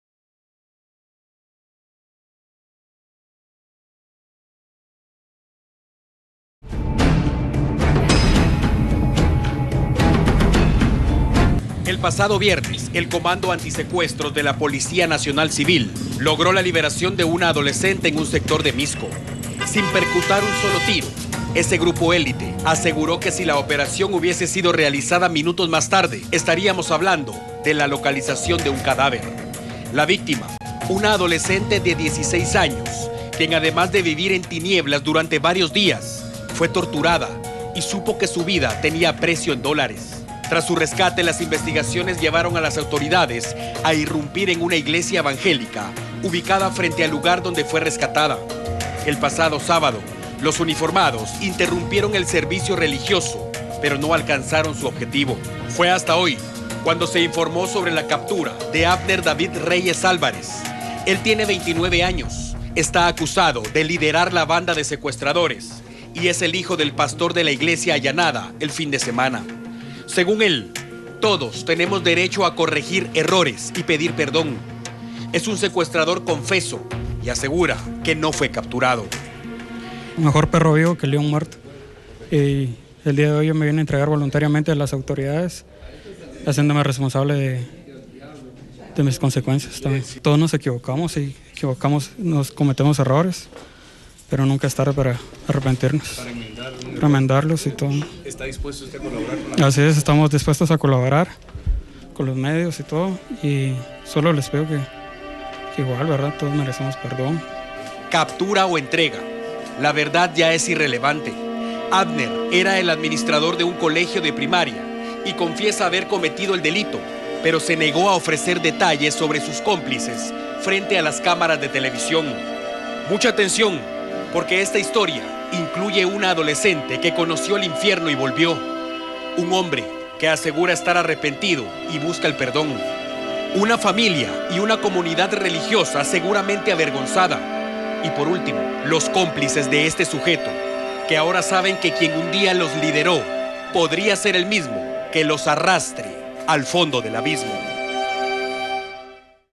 T13 - HIJO DE PASTOR CAPTURA POR SECUESTRO
DEL SENO DE UNA FAMILIA QUE PUBLICAMENTE PREDICA LA PALABRA EN SU PROPIA IGLESIA Y FORMA A ESTUDIANTES EN UN COLEGIO TAMBIEN DE SU PROPIEDAD. SURGIO UN MIENBRO DEDICADO A MULTIPLICAR SU RIQUEZA MATERIAL.